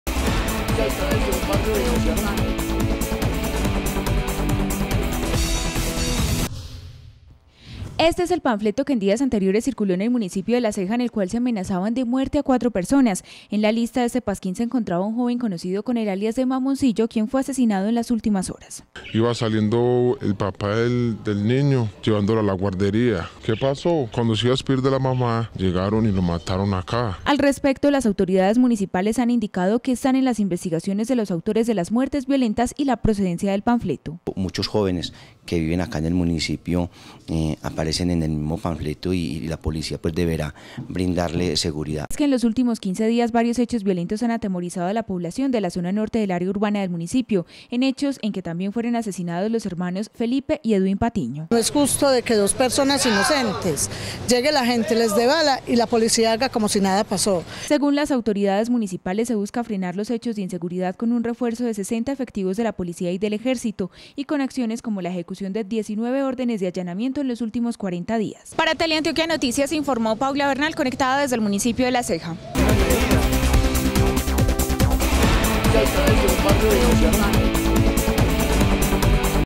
Asesinado Joven En La Ceja Que Había Sido Intimidado A Través De Un Panfleto| Teleantioquia Noticias
En las ultimas horas fue asesinado un joven en el municipio de La Ceja. La víctima había sido intimidada a través de un panfleto que circuló hace aproximadamente una semana en esa población.